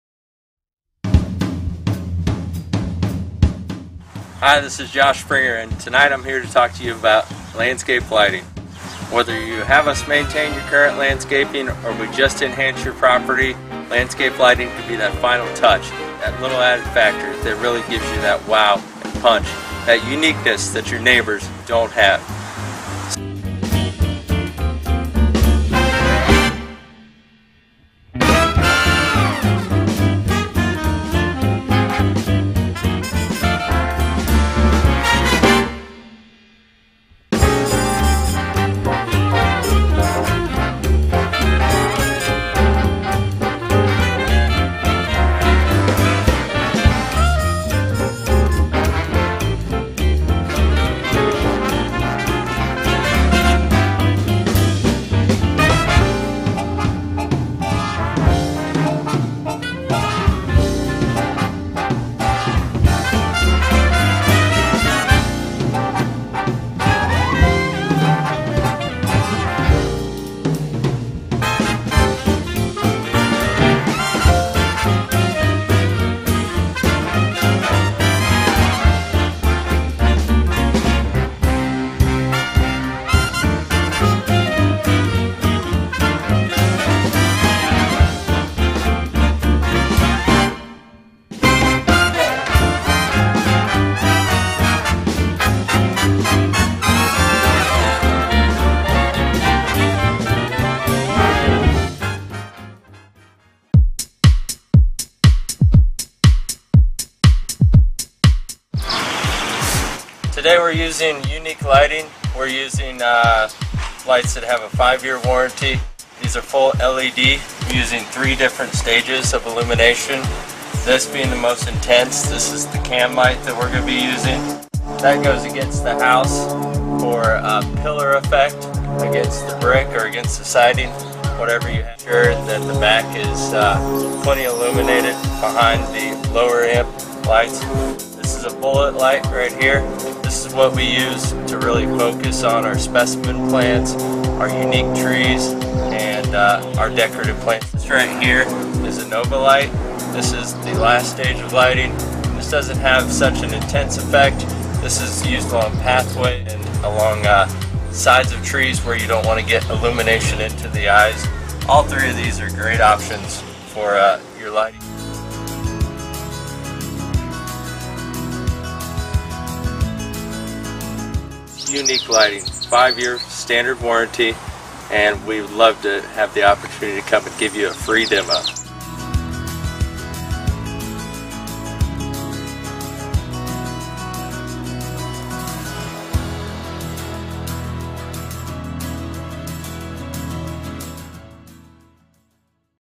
Property Pros Lighting Video
Find out all of the reasons that proper lighting will add tremendous value to your home.